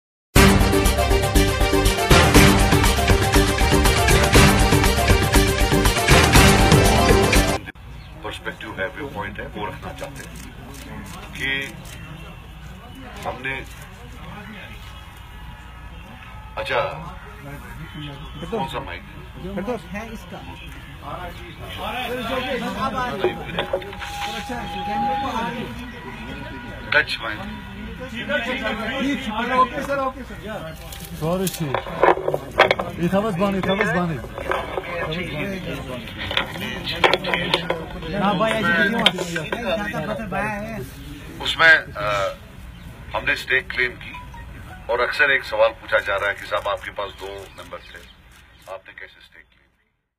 Staked Claim as we have numbers in J&K, says Sajjad Lone By Agencies
Staked Claim as we have numbers in J&K, says Sajjad Lone

By Agencies

NEW DELHI: Sajjad Gani Lone, chairman of Jammu and Kashmir People’s Conference, on Friday said that he staked claim to form the government in the state as he possesses the required number of legislators in the Assembly.

“We staked the claim as we had the numbers. Had there been a floor test or had we been told to bring in the numbers, then what is within the purview of the constitution would have happened,” Mr Lone told news agency ANI.

To counter the move to form a government by Congress- People’s Democratic Party (PDP)-National Conference (NC), Mr Lone on Wednesday staked claim to form a government in the state with the support of the 25-legislator strong Bharatiya Janata Party (BJP) and more than 18 other lawmakers.

The People’s Conference Chairman dared the PDP to go to the court if they have the required numbers.

“If they (PDP) are so aggrieved they can go to the court but they will not because they don’t have the numbers,” he said.

Reacting to arch-rivals PDP and NC coming together with the Congress to stake claim to form a new coalition government in the state, Mr Lone said, “We are happy that we got these dynastic parties together.”

A political tussle broke out in Jammu and Kashmir soon after Governor Satya Pal Malik suddenly dissolved the state assembly on Wednesday.